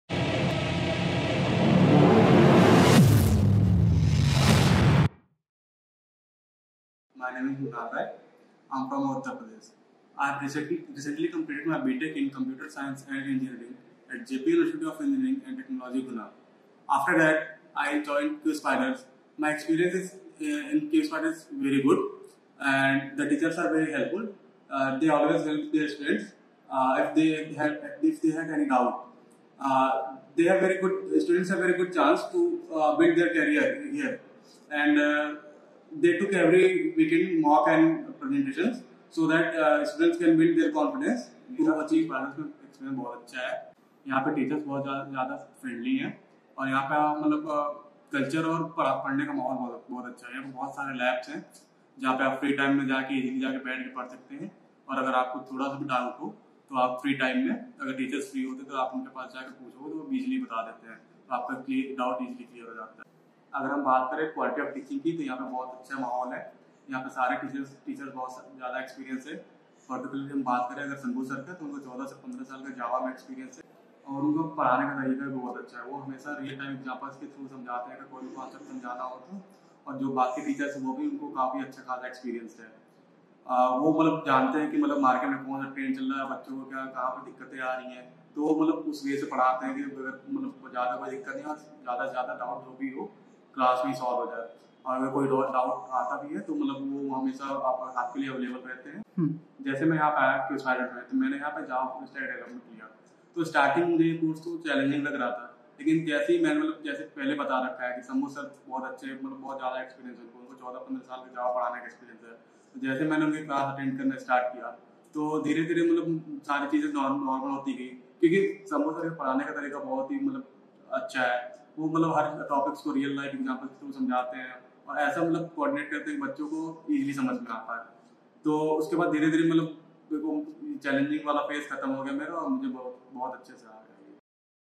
SOFTWARE ENGINEER || JSPIDERS NOIDA || PLACED STUDENT REVIEW
Congratulations Mrinal Rai
Got Placed  as Software Engineer
We Wish All The Best For Your Bright Future